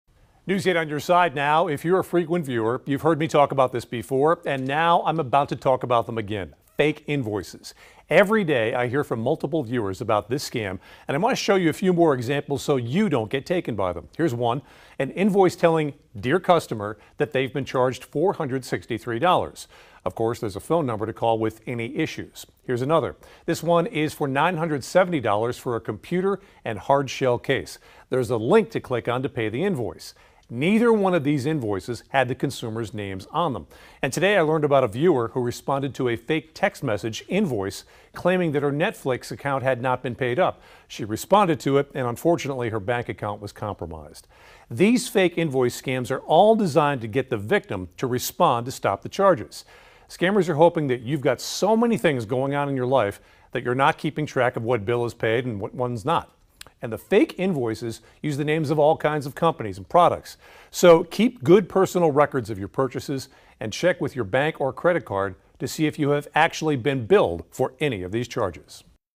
Fraudsters try to trick you into believing you owe money for something
The 8 On Your Side team continues to hear from multiple viewers every day about the fake invoice scam.